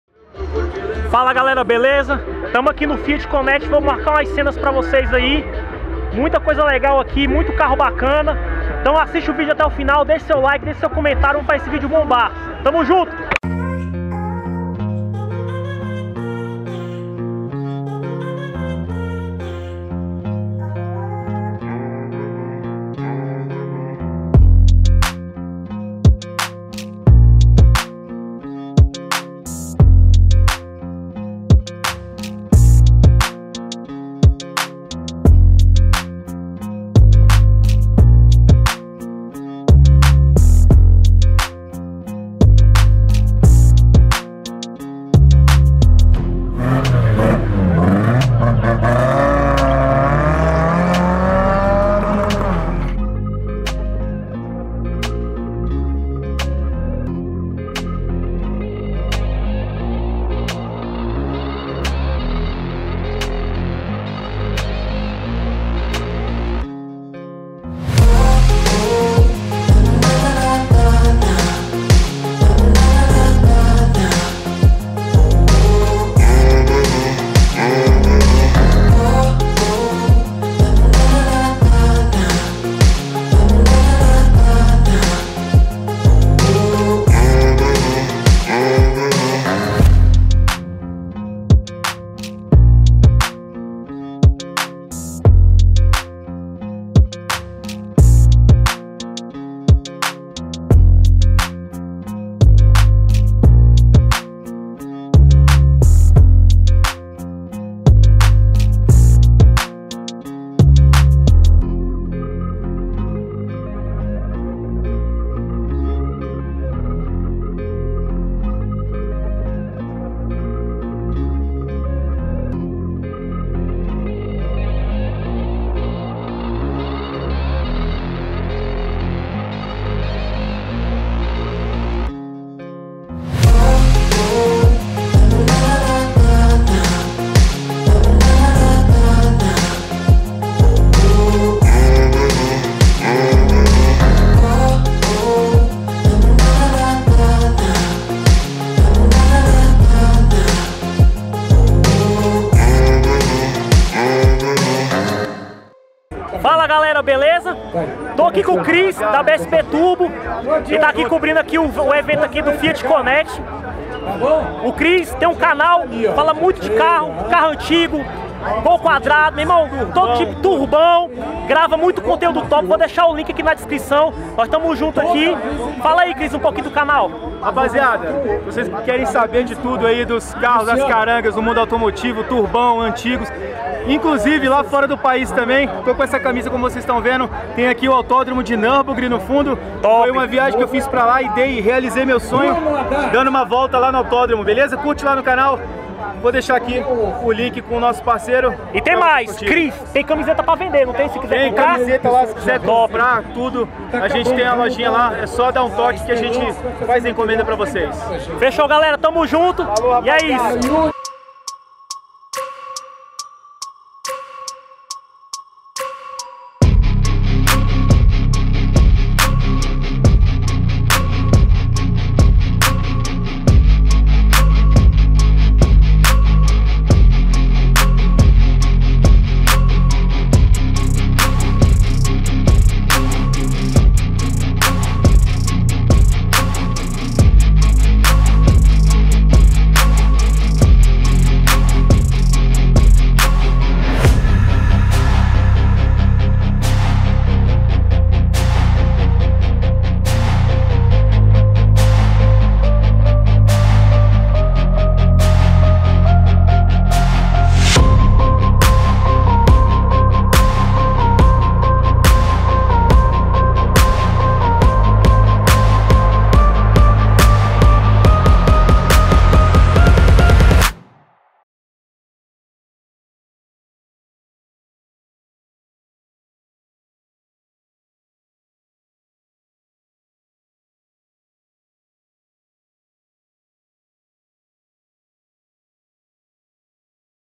O MAIOR ENCONTRO FIAT DO BRASIL
FALA GALERA, Domingão dia 10/04 tivemos mais uma edição do maior encontro fiat do Brasil aqui em Brasília, Fiat Conection realizado na BALI no SAAN, gravamos vários carros e tivemos um bate papo rápido com o Cristiano Brito do Canal BSB Turbo, link abaixo.